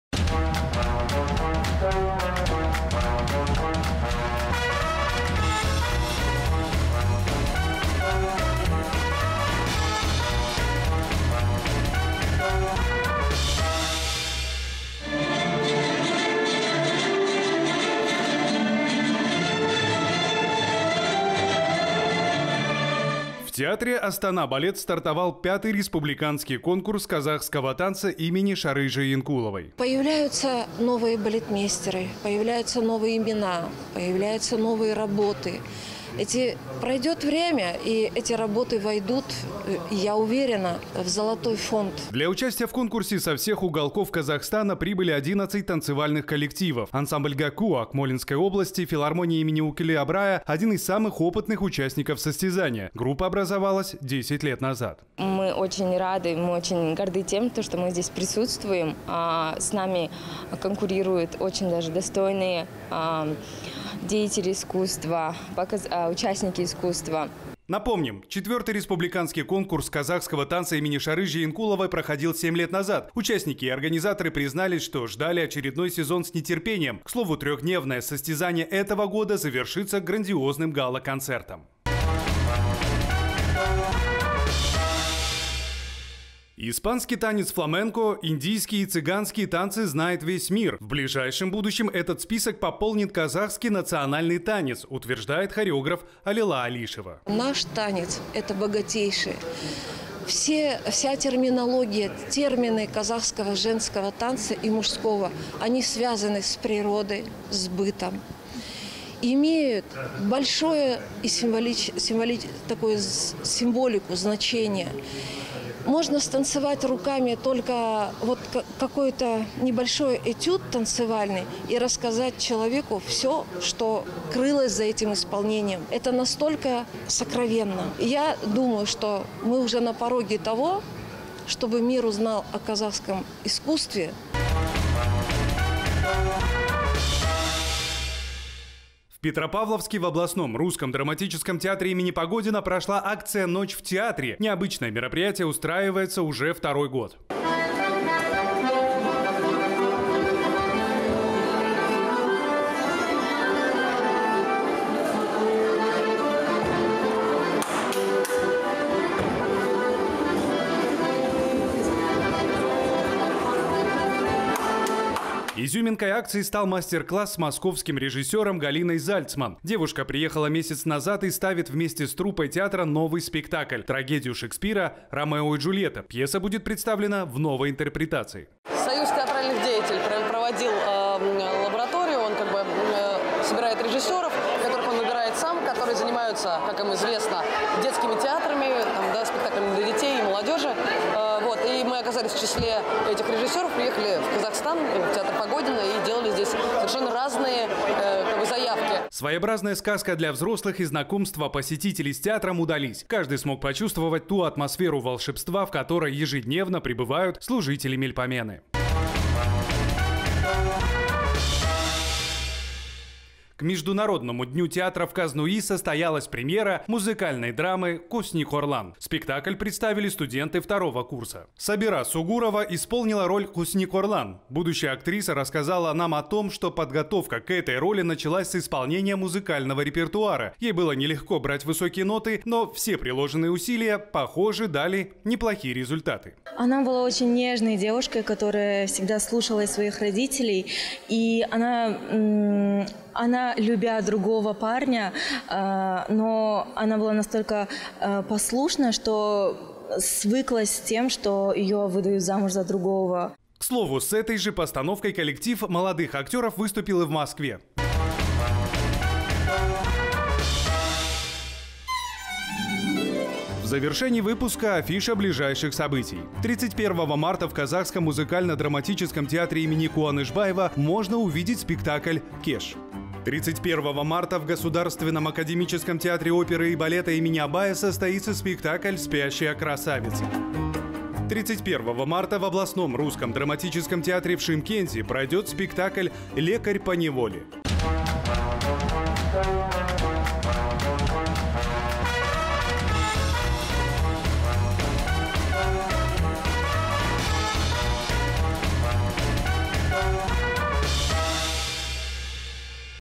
V республиканский конкурс казахского танца
В театре «Астана балет» стартовал V республиканский конкурс казахского танца имени Шары Жиенкуловой.